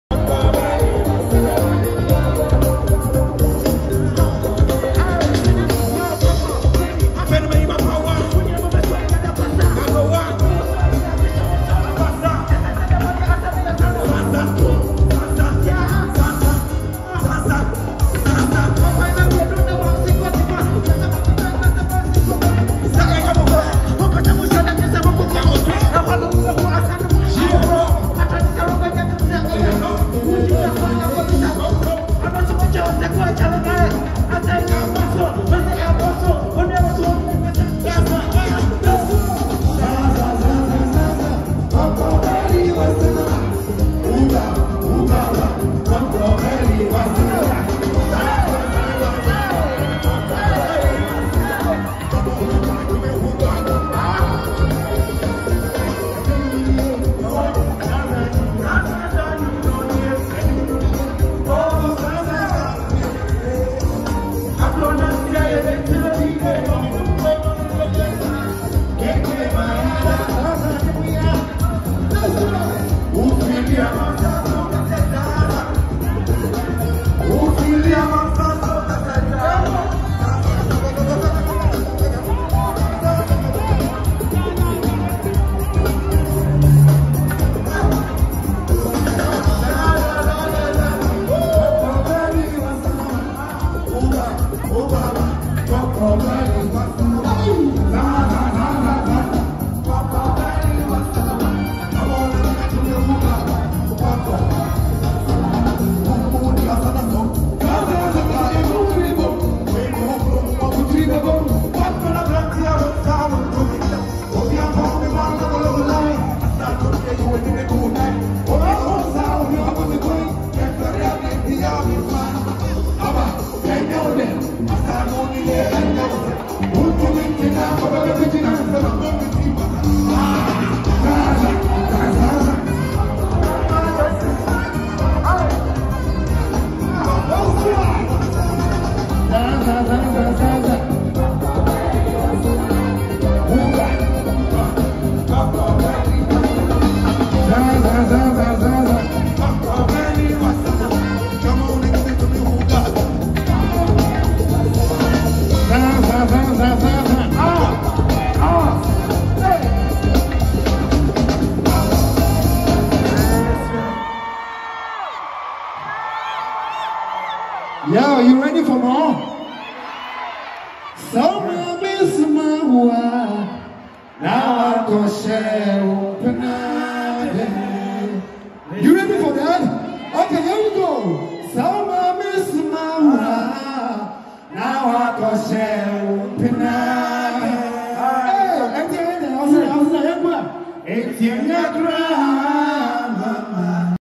TRINITY - Legendary Performance.
KKFosu and the TRINITY live performance that got the world talking.